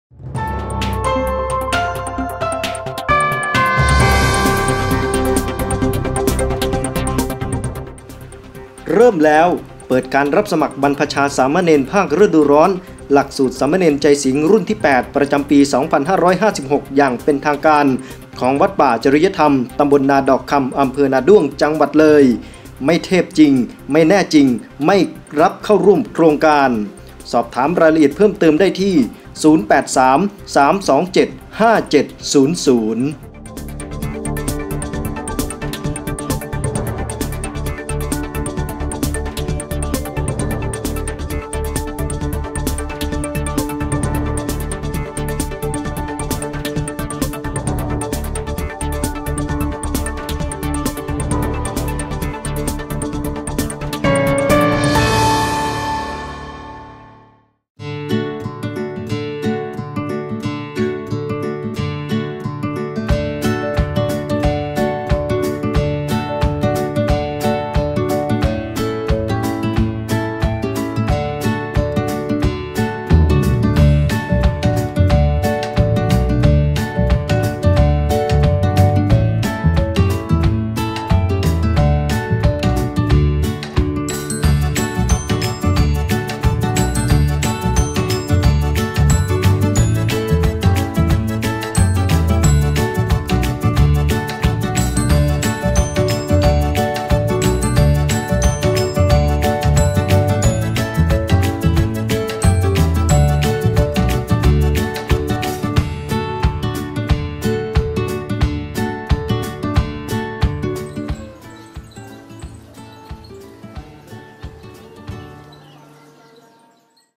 VTR. เด็กวัด
ประชาสัมพันธ์บรรพชาสามเณรภาคฤดูร้อน หลักสูตรสามเณรใจสิงห์ รุ่นที่๘ ประจำปี ๒๕๕๖ วัดป่าจริยธรรม ตำบลนาดอกคำ อำเภอนาด้วง จังหวัดเลย ระหว่าง ๒๕ มีนาคม - ๒๕ เมษายน ๒๕๕๖